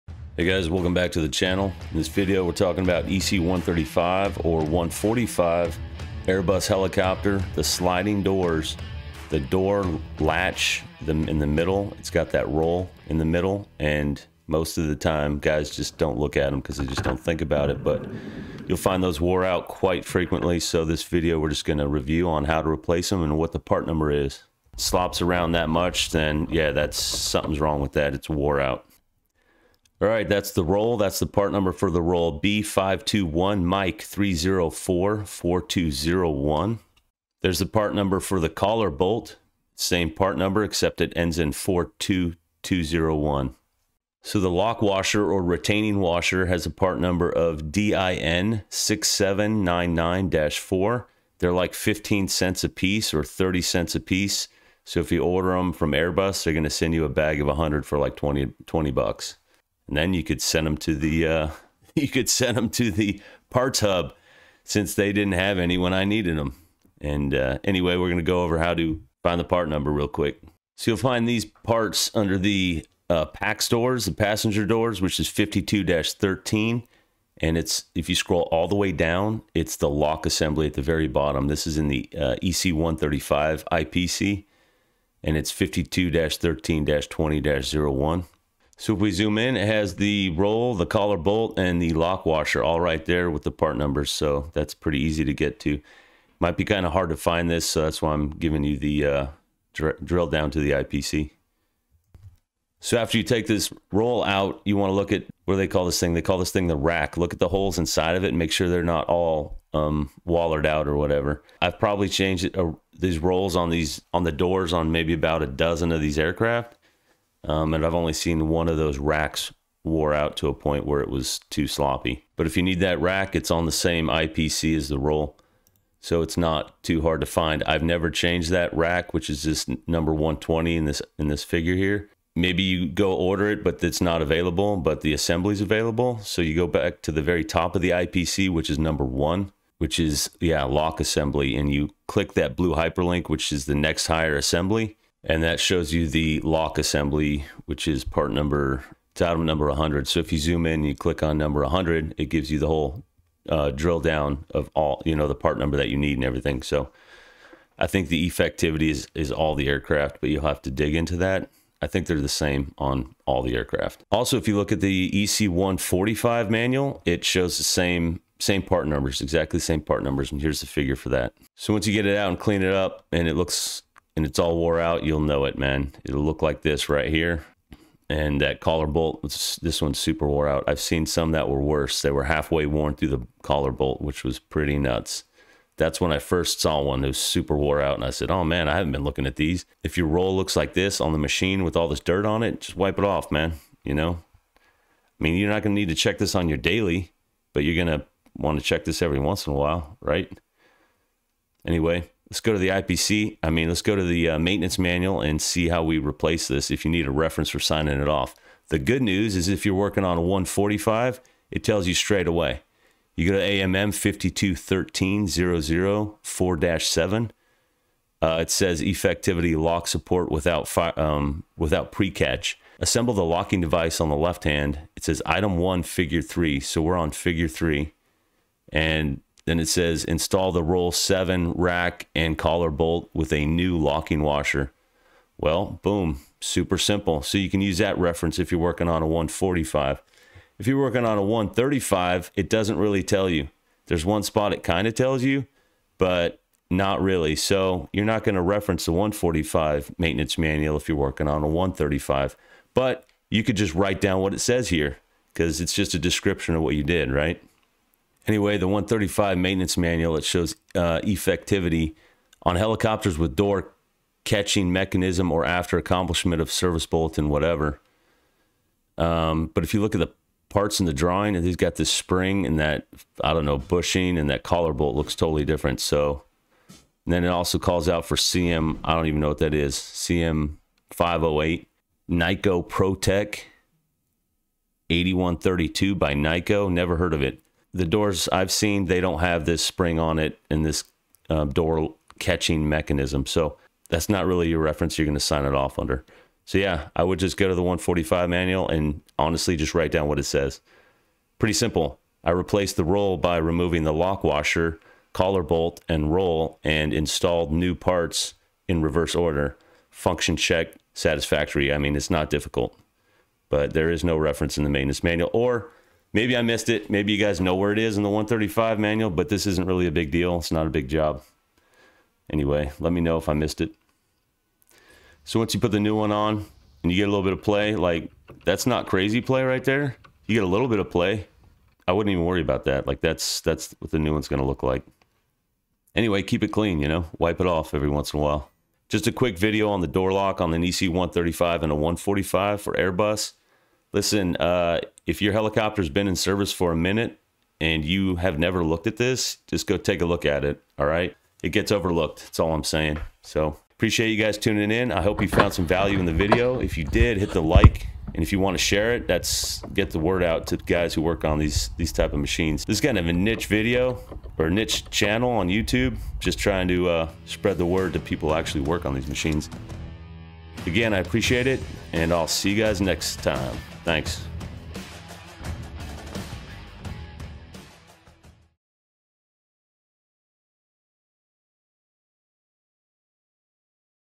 Sliding Door Lock Roll | Airbus Helicopters | EC135 | EC145
In this 6 minute video we look at the sliding door lock/latch mechanism roll and how this item is overlooked frequently.  This is a quick and easy thing to change out.  We review where to find these items in the IPC, both the 135 and 145.  We also review where to find this in the maintenance manual.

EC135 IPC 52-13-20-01
  Items:
130 - Roll - B521M3044201
140 - Collar Bolt - B521M42201
150 - Retaining washer - DIN6799-4

EC145 IPC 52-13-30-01item 10, 20, and 30.  Same Part numbers as EC135.

Maintenance manual instructions are found in the EC145 AMM 52-13-00, 4-7

This is a very simple job.  If your machine has been flying for a long time and you have not looked at this ever, then take a quick look.

I hope you found value in this video.  Consider sharing these videos to help the other guys out there.

Regards,
-Helicopter Maintenance Help Desk